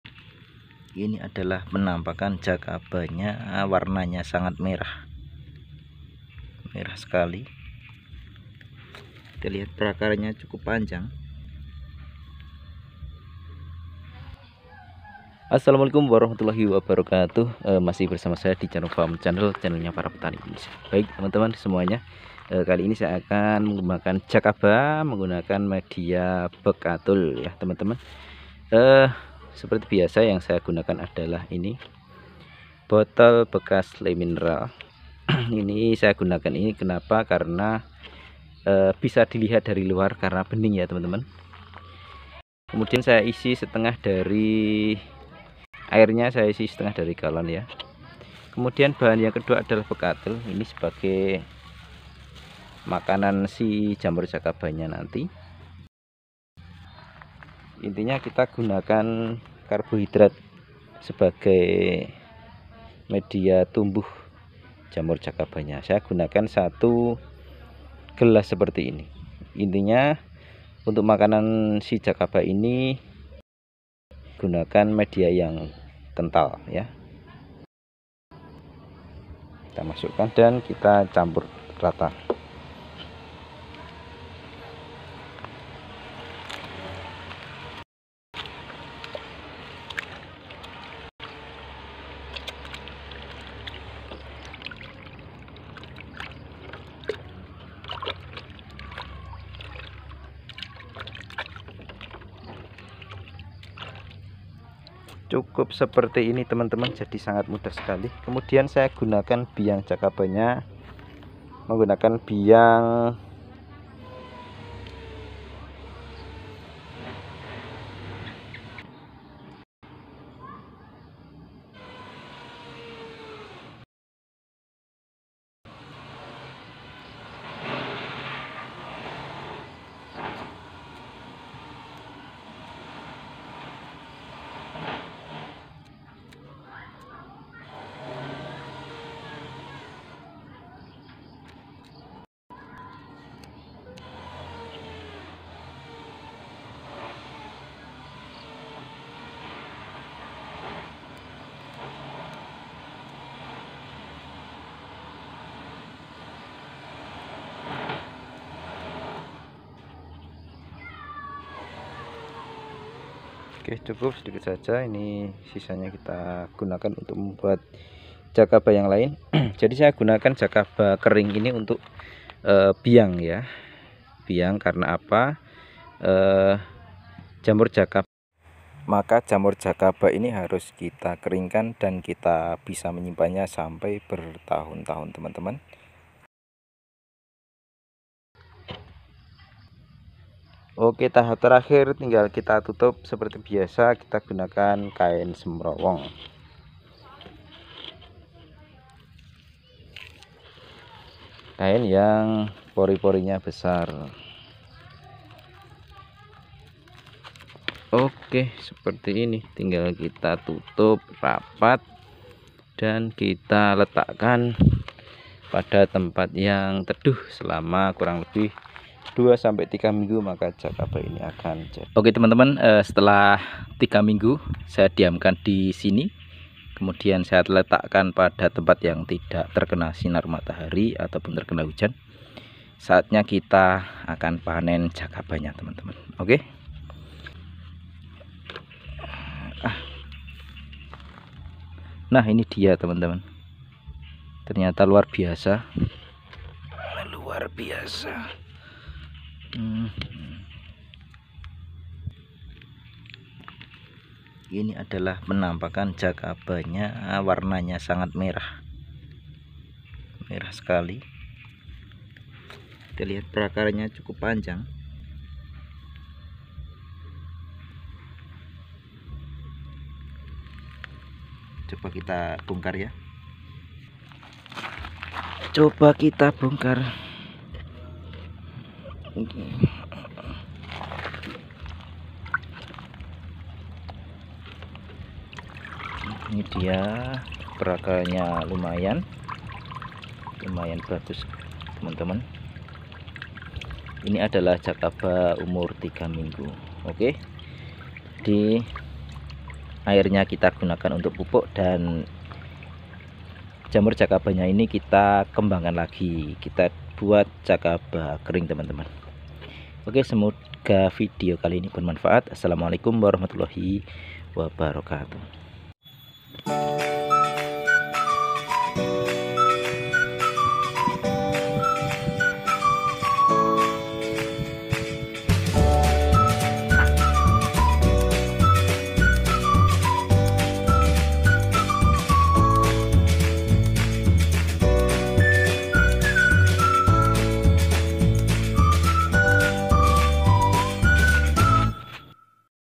Membuat Jakaba dengan media bekatul / dedak || urban gardening
membuat jakaba dengan media bekatul sangat mudah di lakukan petani